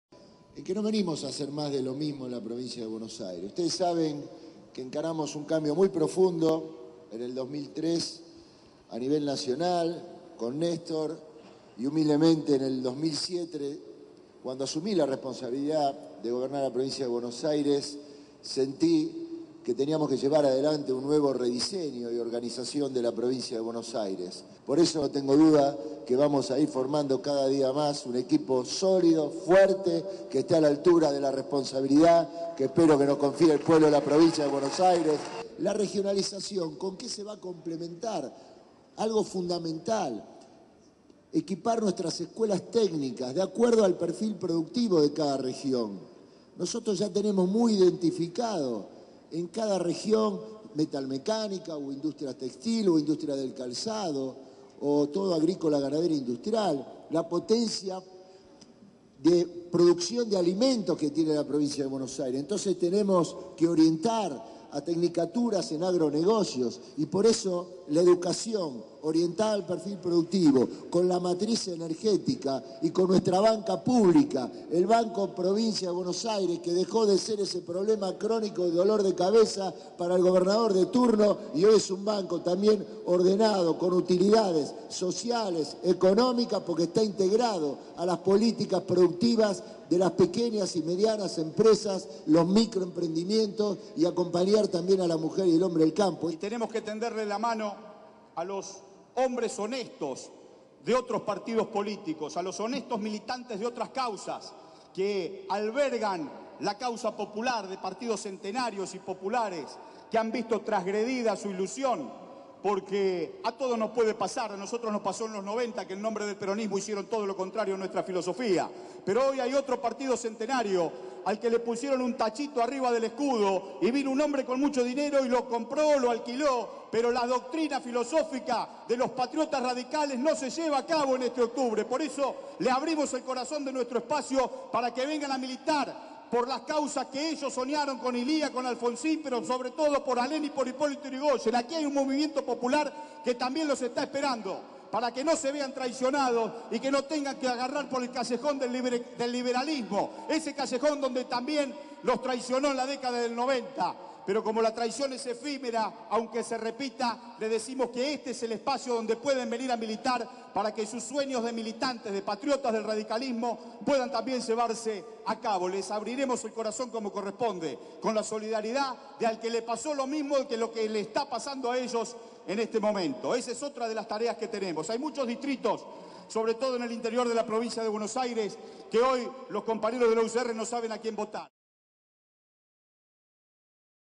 Scioli y Mariotto presentaron un plan de regionalización con acento productivista
La fórmula bonaerense del FpV encabezó esta tarde un acto en la Universidad Nacional de San Martín (UNSAM), donde presentaron un plan de regionalización de la provincia de Buenos Aires.
El gobernador bonaerense y candidato a la reelección por el Frente para la Victoria (FpV), Daniel Scioli, calificó hoy de "sólida" y "convincente" la fórmula que integra junto a Gabriel Mariotto, con quien dijo "no venimos a hacer más de lo mismo, sino a imprimirle un paso más al crecimiento de la provincia".
Durante el acto, Scioli aseguró que junto con Gabriel Mariotto "no venimos a hacer más de lo mismo, sino a imprimirle un paso más al crecimiento de la provincia" y califico a la fórmula que integra con el titular de la Autoridad Federal de Servicios de Comunicación Audiovisual (AFSCA) como "sólida, convincente y a la altura de las circunstancias".
En ese sentido, Scioli afirmó que "estamos para construir una provincia moderna, con un Estado inteligente y tenemos como desafío lograr dotar de valor agregado a nuestras materias primas, porque el primer paso que di en la provincia fue alcanzar el orden fiscal y financiero".
Mariotto, por su parte, cuestionó la alianza entre el radicalismo y el peronista disidente y candidato a la gobernación, Francisco De Narváez, para lo cual se refirió al electorado radical de la provincia.
"A la UCR sólo le faltó poner un tachito de venta en el techo", dijo el titular de la AFSCA, sobre el entendimiento con el empresario colombiano e invitó a los radicales a "debatir y discutir el futuro dentro de nuestro proyecto nacional y popular".
El funcionario señaló además que "con Scioli estamos pensando la provincia del futuro y con las raíces de la universidad bien profundas para poder replicar los modelos de inclusión".
Por último, Mariotto subrayó que "sabemos que no vamos a fallarle a nuestra historia y a nuestro destino", al tiempo que vaticinó: "Reventaremos las urnas en (los comicios de) agosto y en octubre".